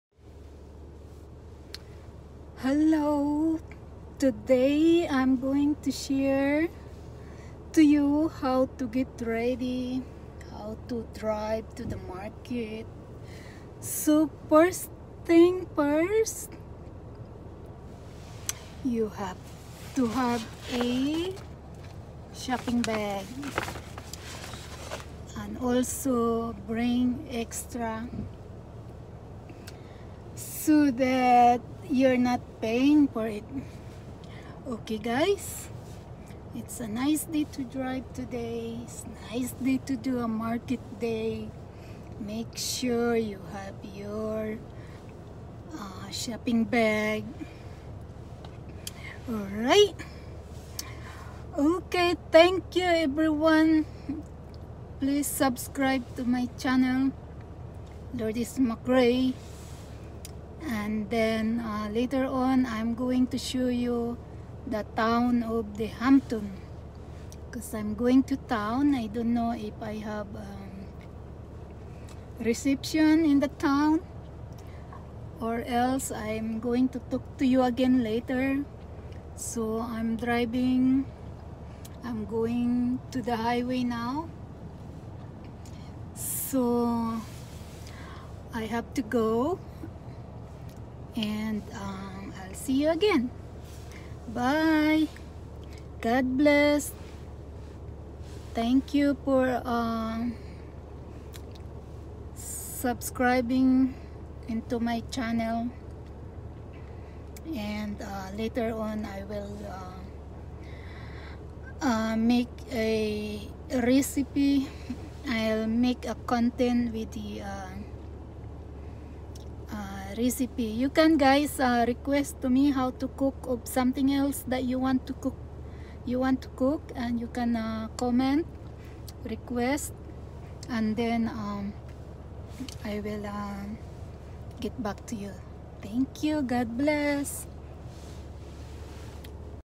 How to get Ready to do food Shopping
How to get Ready to do Food Shopping. Make Sure to Bring your Own Shopping Bags Leave Some Inside your Car